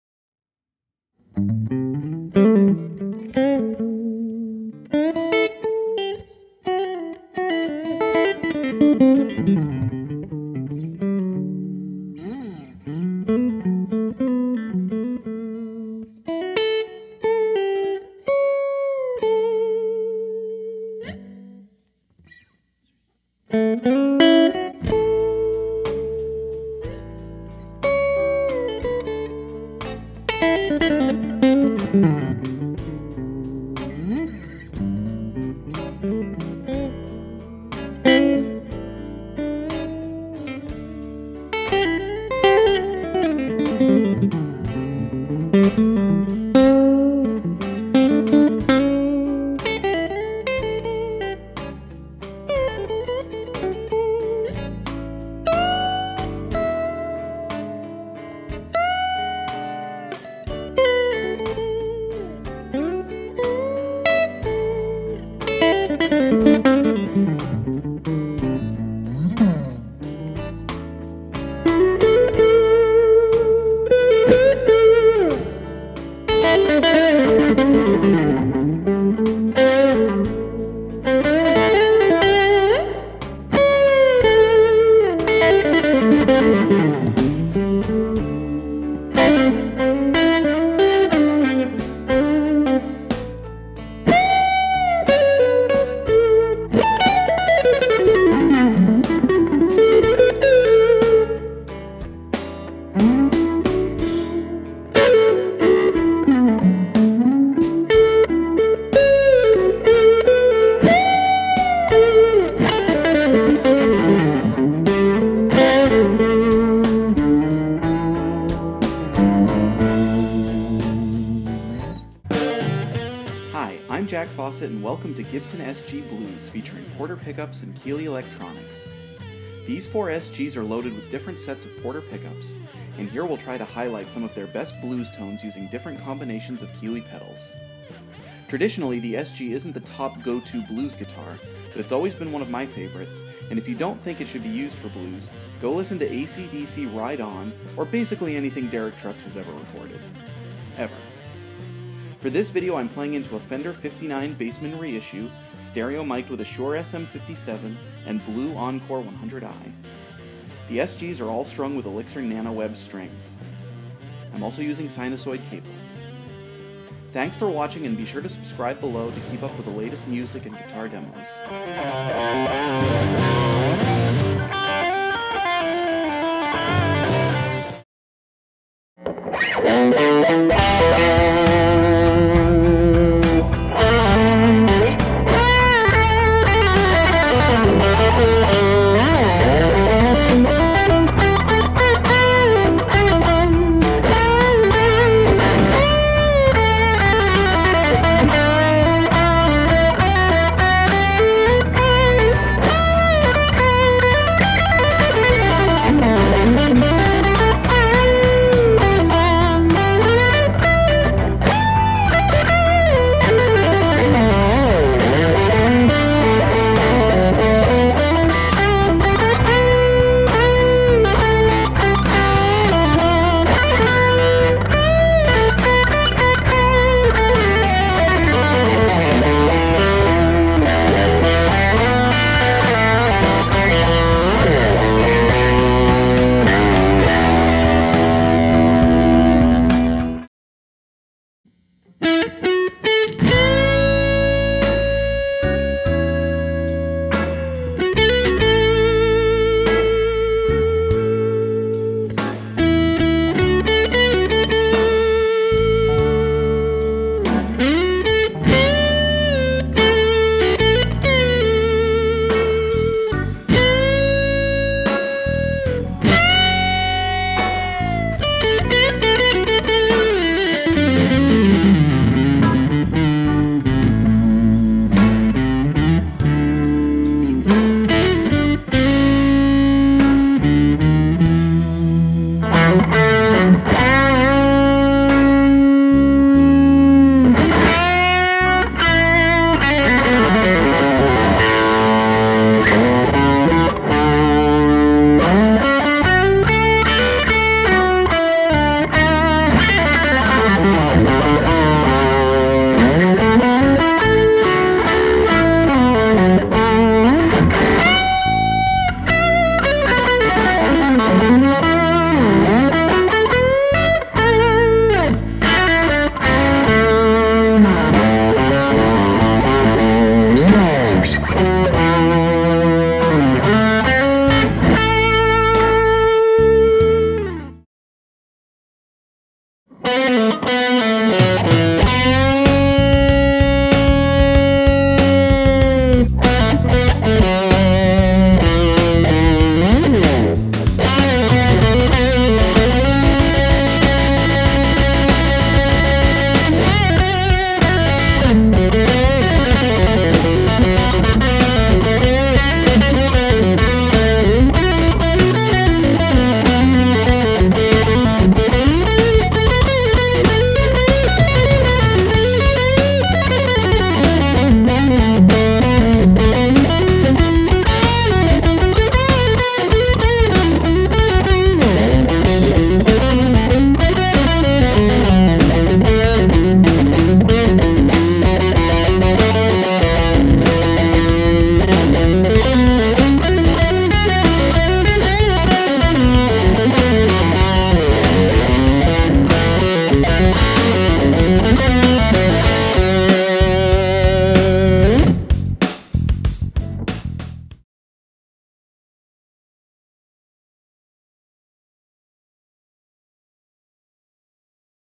Gibson SG Blues Jams
Gibson SG Blues
Gibson SG Standard with Porter Clear Humbuckers
Gibson SG Classic with Porter Smooth & Classic P90s
Gibson SG Custom with Porter Anthem PAF Humbuckers
Gibson SG Standard with Porter Overdrive Humbuckers
Fender 59 Bassman Reissue
Keeley DS9 Distortion Pedal
Keeley 1962 Overdrive Pedal
Keeley Aurora Reverb Pedal
Keeley Red Dirt Overdrive Pedal
Keeley Dynatrem Tremolo Pedal
Sinasoid Cables
Shure SM57 Mic
Blue Encore 100i Mic
Presonus iTwo Interface

and The Backing Track Collective